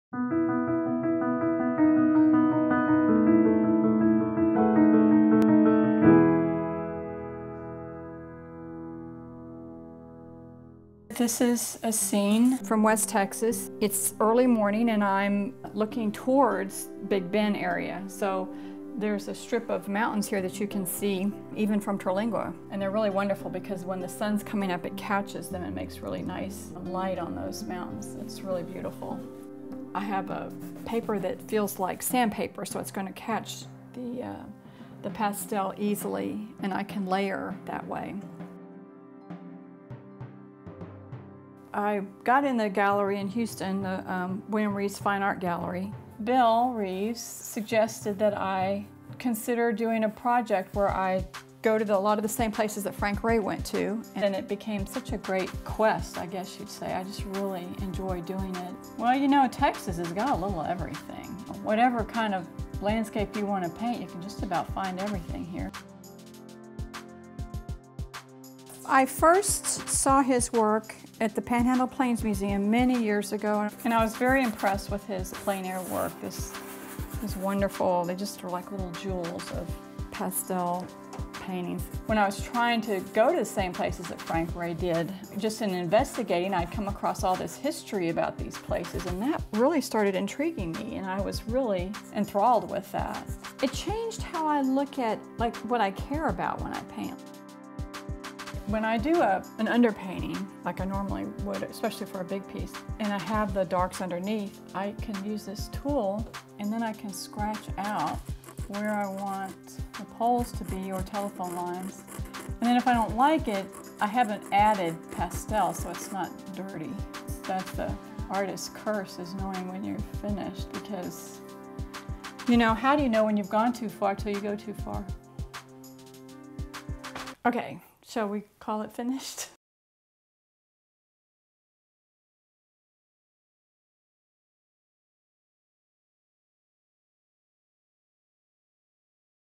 Jeri Salter, Pastel Artist
Texas artist Jeri Salter is a modern pastel painter following the heritage of Frank Reaugh. Learn what Salter loves about Texas landscapes and how she re-traced Reaugh’s process a century later.